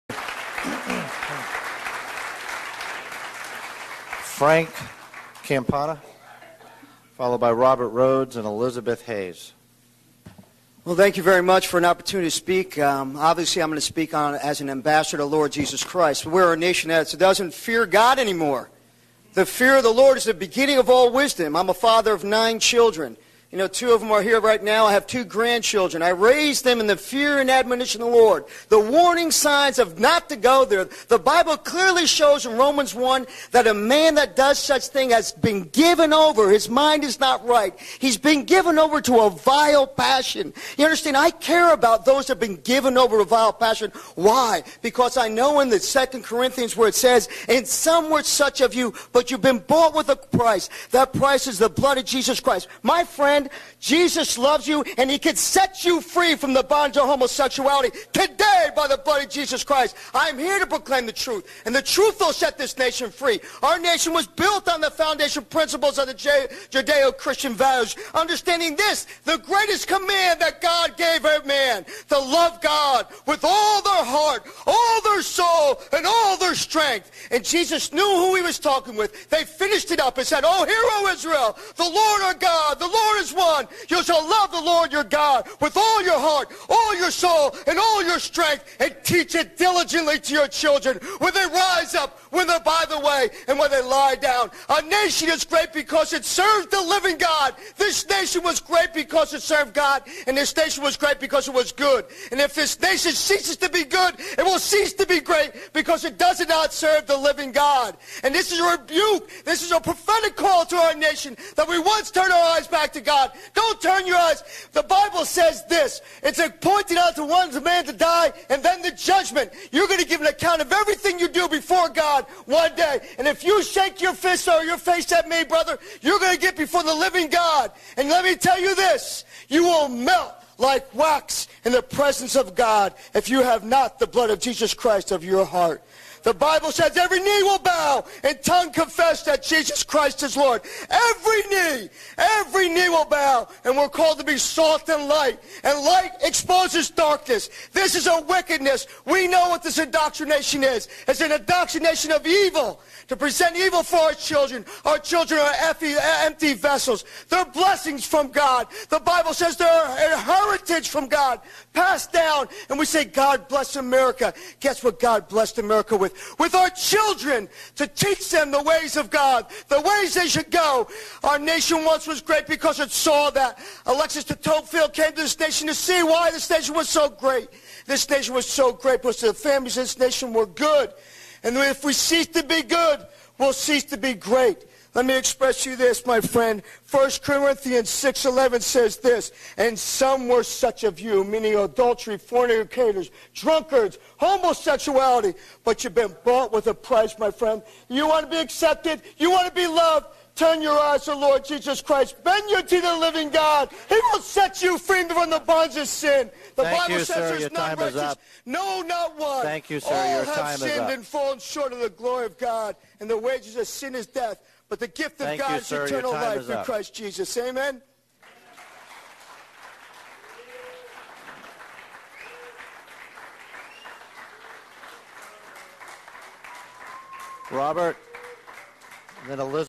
Frank's comments at the February school board meeting
Back on February 23rd of this year the Brevard County school board held a special meeting. More than a hundred Christians showed up to make public comments against the LGBT anti-discrimination and gender neutral restroom agenda being proposed to the school board. This is a clip from my friend and brother in Christ, Frank Campana, making his comments against this LGBT agenda.